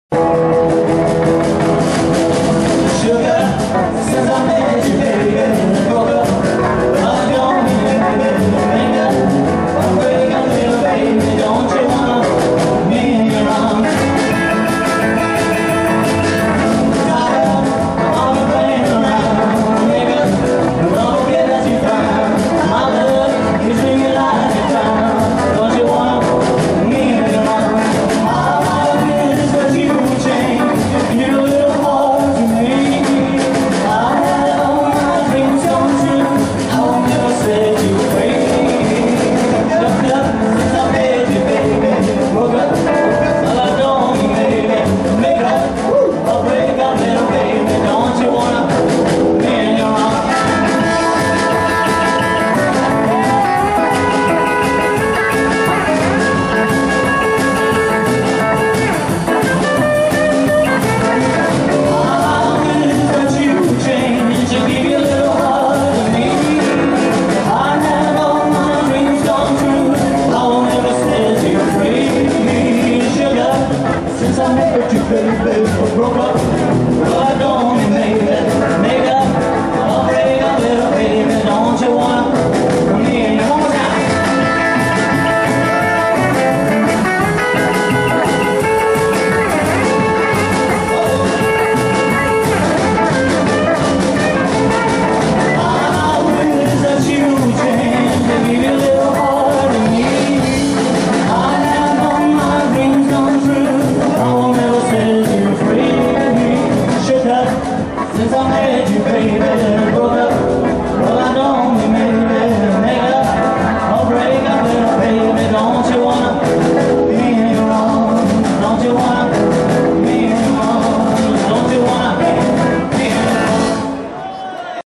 The SHOOKUPS HEMSBY 48
American band The Shookups delivering sugar at Hemsby Rock'n Roll Weekender 48, filmed by uwe40seven